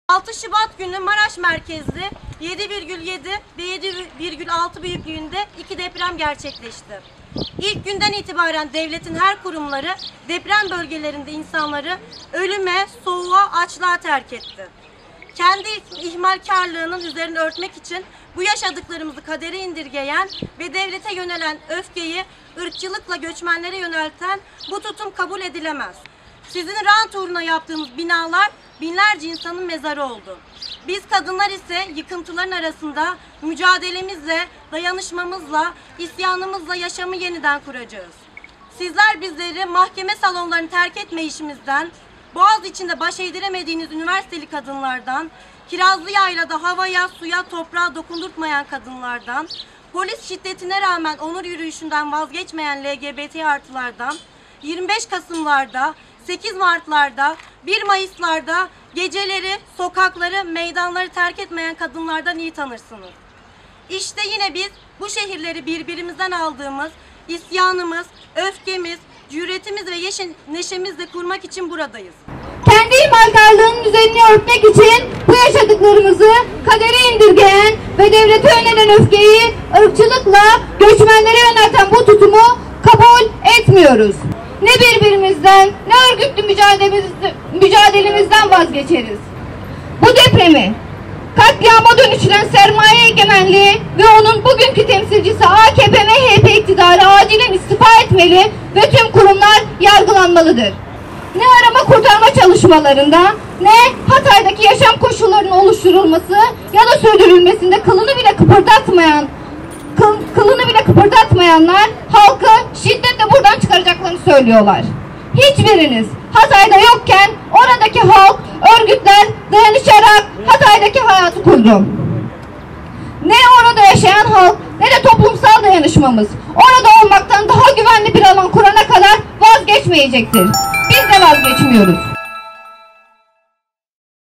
"Yıkılan şehirleri kadın dayanışmasıyla yeniden kuracağız"
8 Mart Dünya Kadınlar Günü’ne ilişkin açıklama yapan kadınlar, yıkılan şehirleri ve yaşamı kadın dayanışmasıyla yeniden kuracaklarını, hiçbir yere gitmeyeceklerini belirtti.